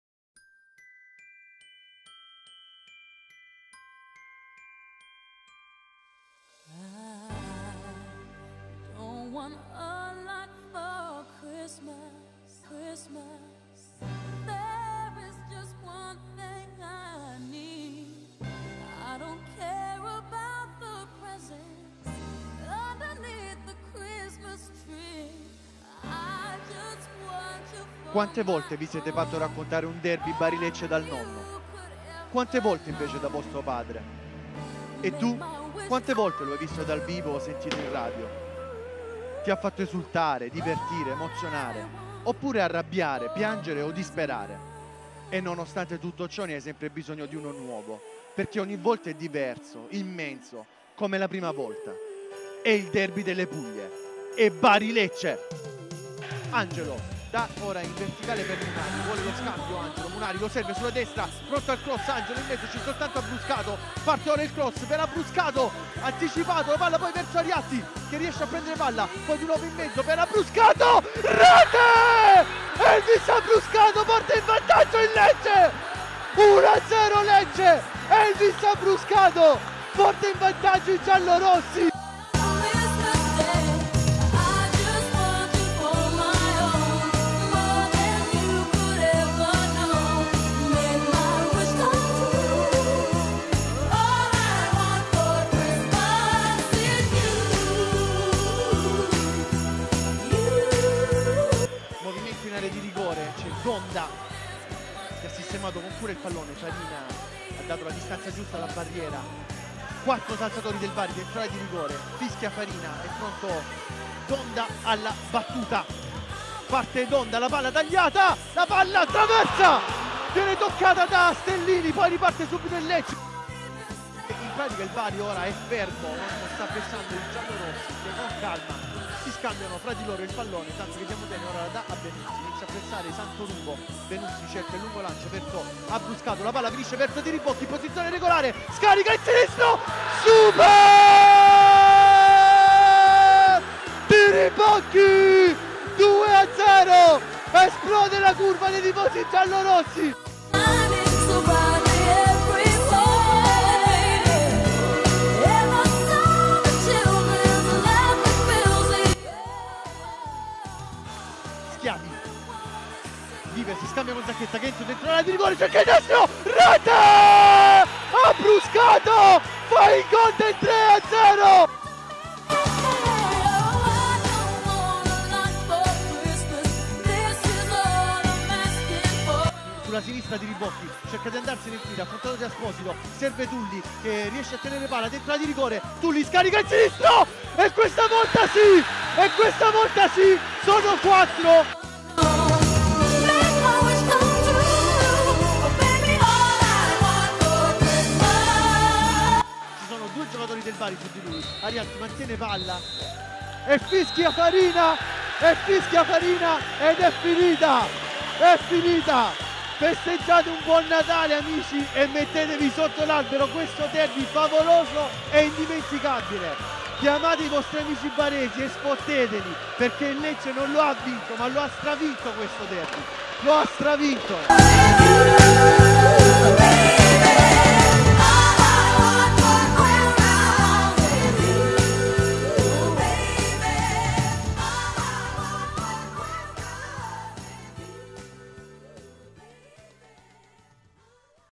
22.12.07 Bari-Lecce 0-4: apoteosi giallorossa al San Nicola
La storica vittoria del Lecce di Papadopulo in casa dei cugini